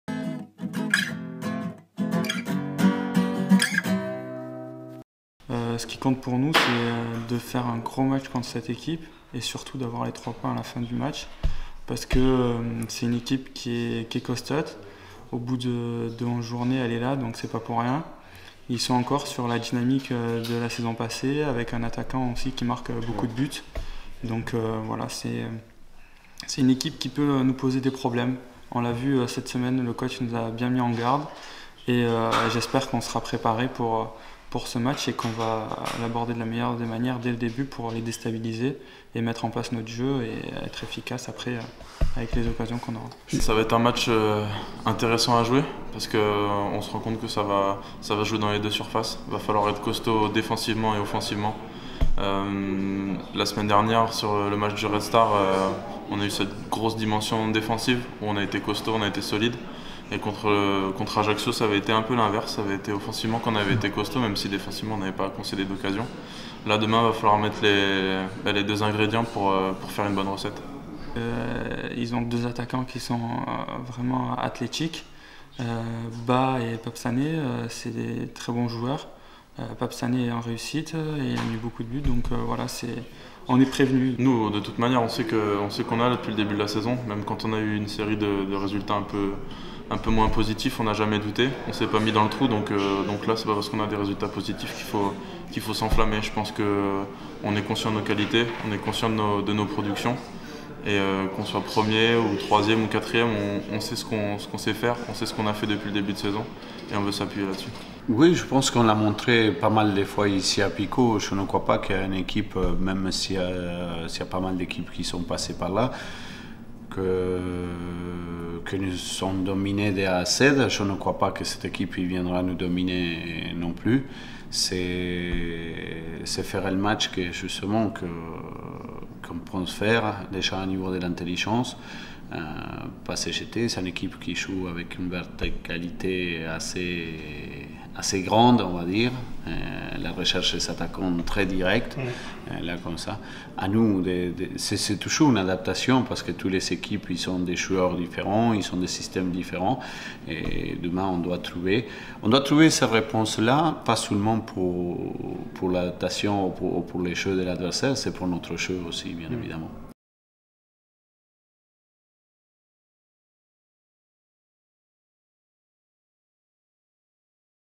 Point presse avant ASNL-Bourg en Bresse
L'ASNL accueille vendredi 23 octobre au stade Marcel Picot l'équipe de Bourg-en-Bresse avec l'objectif de poursuivre sa bonne série, mais il faudra faire sans Youssouf Hadji, forfait.

Au regard du classement, cet ASNL-Bourg représente une affiche puisqu’il opposera le deuxième du classement au quatrième, la meilleure défense du championnat face à la meilleure attaque menée par Pape Sané auteur de huit buts depuis le début de la saison. Mais les Bressans, avec 17 buts encaissés, possède également la plus mauvaise défense de Ligue 2, bref les buts sont souvent ont rendez-vous avec Bourg-en-Bresse à l'image de leurs deux derniers matchs soldés sur des scores de 3-2 à Clermont et une victoire la semaine dernière à domicile 4-2 face à Nîmes.